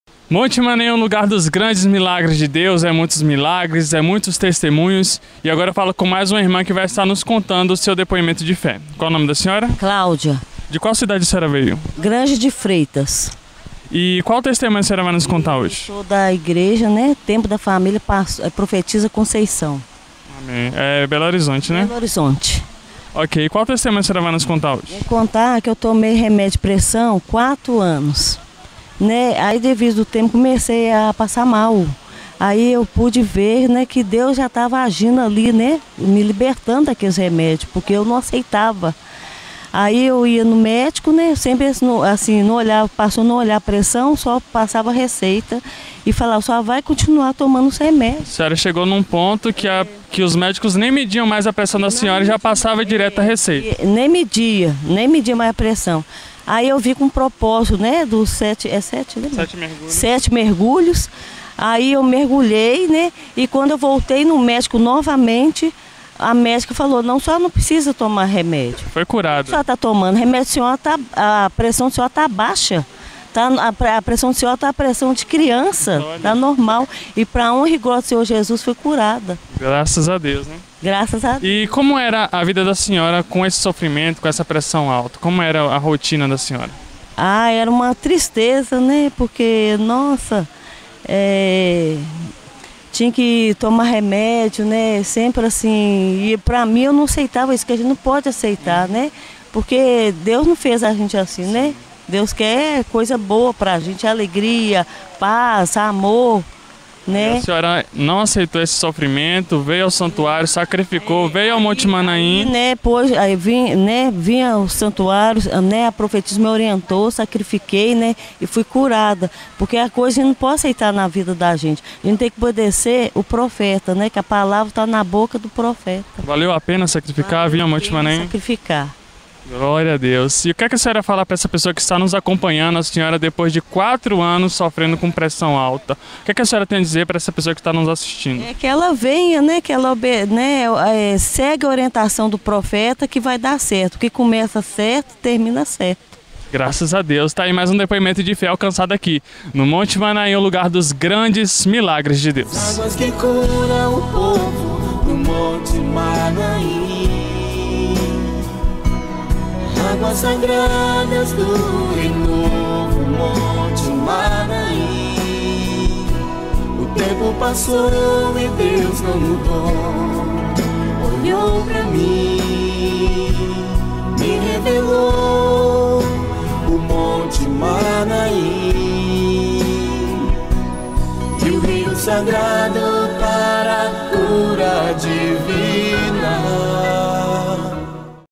Monte Maanaim: Claudia - Sofreu durante 4 anos com pressão alta
Entre na caravana da fé rumo ao Monte Maanaim - O lugar dos grandes milagres de Deus
🙏🏼 Ouça a ORAÇÃO DA MADRUGADA diariamente às 3 horas da manhã